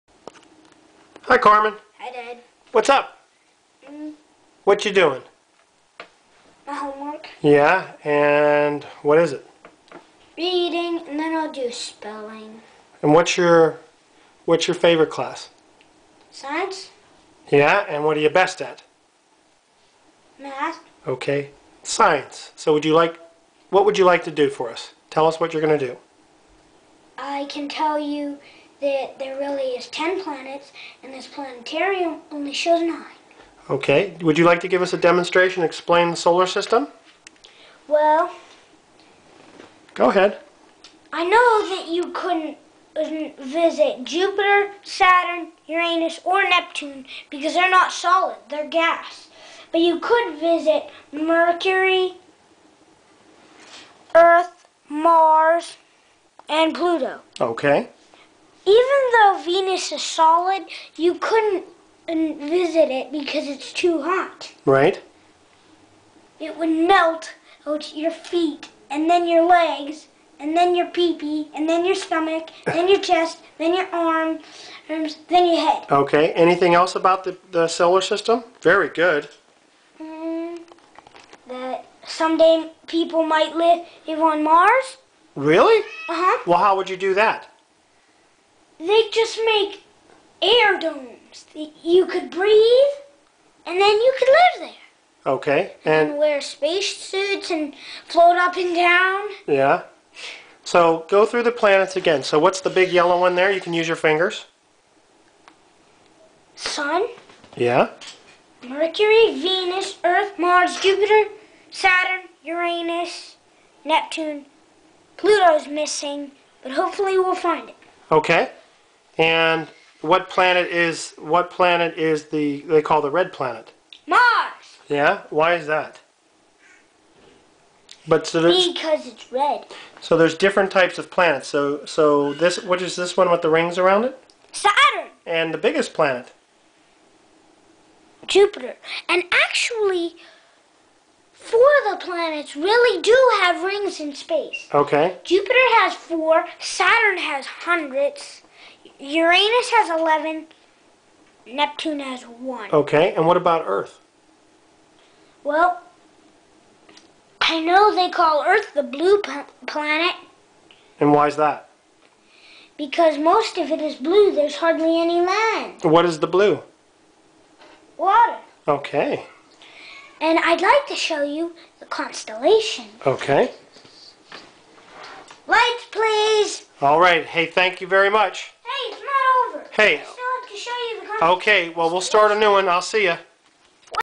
Carmen explains the Solar System
Carmen Michael Cavanagh an 8 year old scientist explains the solar system with his unique view.